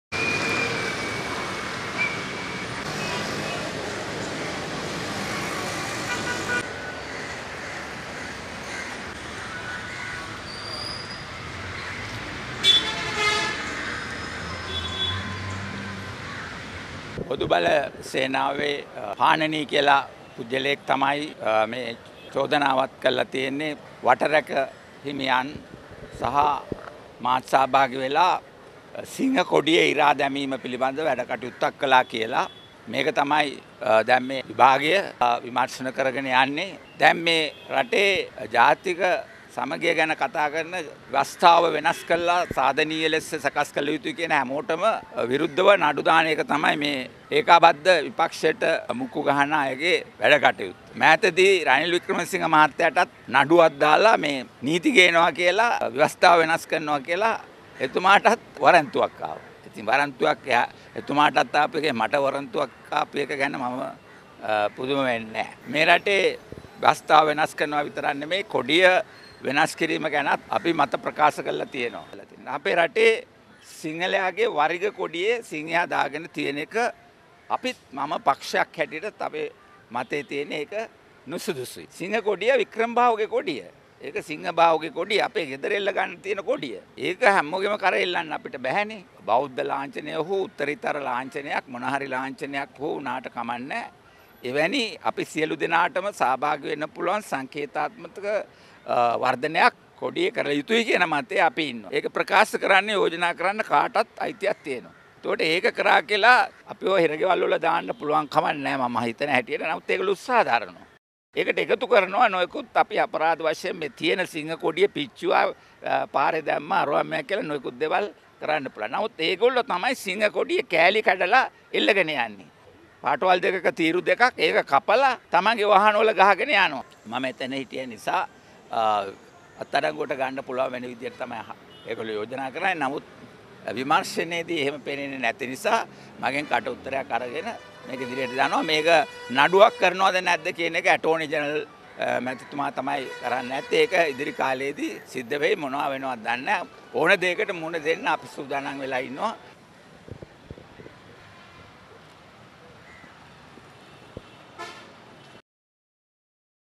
Probe against Vickramabahu for 'abusing Sri Lanka national flag'
ශ්‍රී ලංකාවේ ජාතික කොඩියට අගෞරව කළ බවට කෙරුනු පැමිණිල්ලක් සම්බන්ධයෙන් නව සම සමාජ පක්ෂයේ ප්‍රධාන ලේකම් වරයාට විරුද්ධව පරීක්ෂණ ආරම්භ කර තිබේ.

“පවතින ව්‍යවස්ථාව වෙනස් කර සාධනීය ලෙස සකස් කළ යුතු බවට යෝජනා කරන යට  විරුද්ධව නඩු දැමීම මහින්ද රාජපක්ෂ ගේ අනුගාමිකයන්ගේ කටයුත්ත වී තිබෙන,” බව පරීක්ෂණ පවත්වන  කිරුලපොන පොලිසියට කටඋත්තරයක් දීමෙන් අනතුරුව ආචාර්ය වික්‍රමබාහු කරුණාරත්න මාධ්‍යවේදීන්ට කියා සිටියේය.